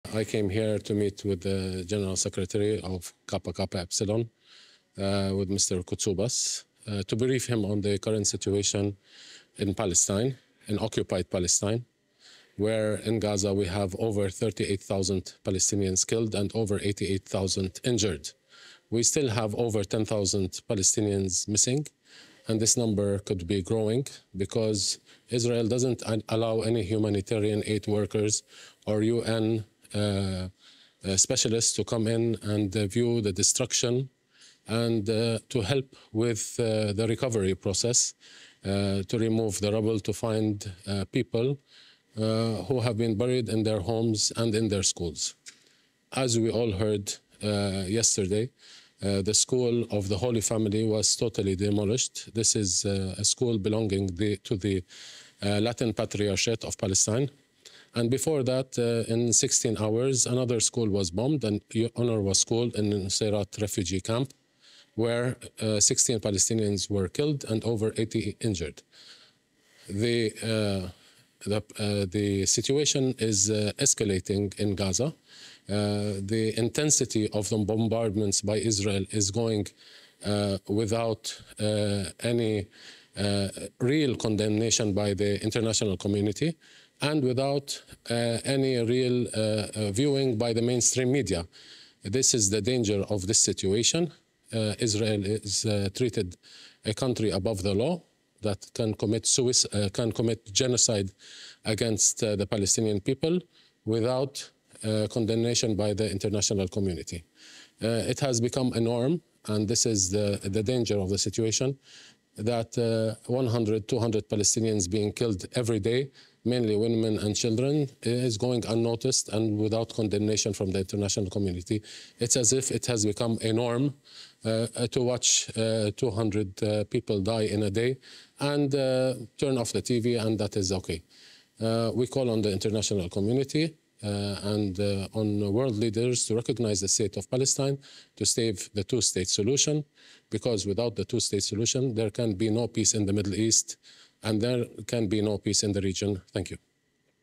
Δήλωση του πρέσβη της Παλαιστίνης μετά τη συνάντηση με τον Δ. Κουτσούμπα
Στη συζήτηση που ακολούθησε ο πρέσβης της Παλαιστίνης ενημέρωσε την αντιπροσωπεία του ΚΚΕ για τα εγκλήματα του Ισραήλ στη Γάζα και τη γενοκτονία κατά του Παλαιστινιακού λαού, ο οποίος προβάλλει σθεναρή αντίσταση τόσο στη Γάζα, όσο και στη Δυτική Όχθη που δέχεται κι εκεί δολοφονικές επιθέσεις του ισραηλινού - κατοχικού στρατού και των εποίκων. Σημείωσε ιδιαίτερα την ανάγκη να φτάσει άμεσα η ανθρωπιστική βοήθεια και από την Ελλάδα και από άλλες χώρες. Ο πρέσβης ευχαρίστησε το ΚΚΕ για την αμέριστη αλληλεγγύη του στον Παλαιστινιακό λαό και την αποφασιστική καταδίκη του κατοχικού κράτους του Ισραήλ.